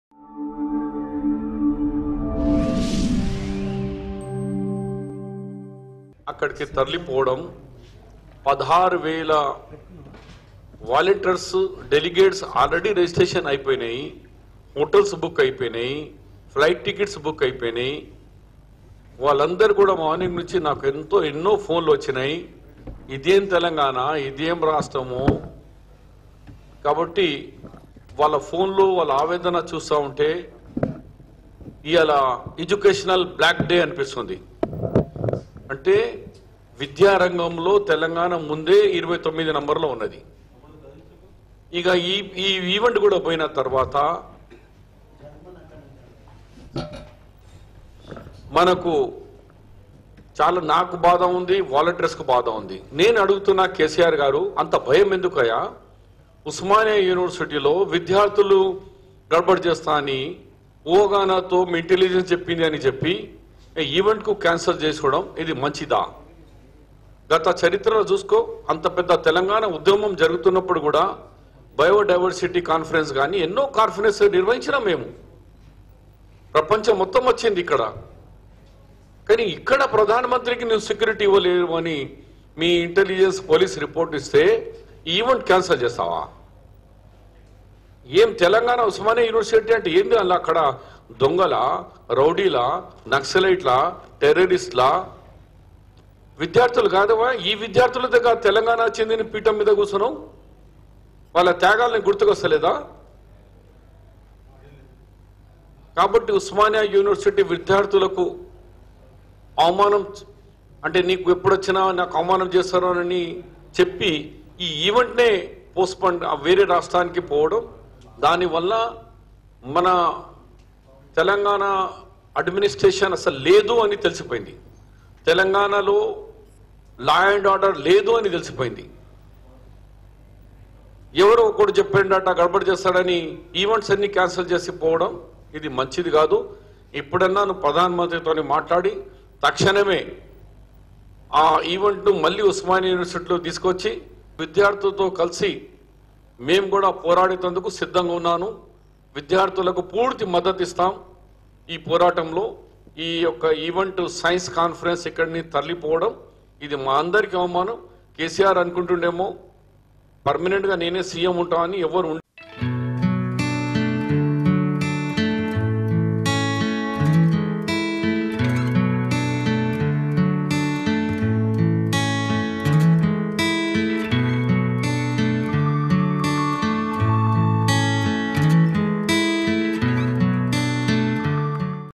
T Congress Shabbir Ali Fires On CM KCR | KCR Comments On Osmania University Students | IndionTvNews
T Congress Shabbir Ali Fires On CM KCR over KCR's Comments On Osmania University Students.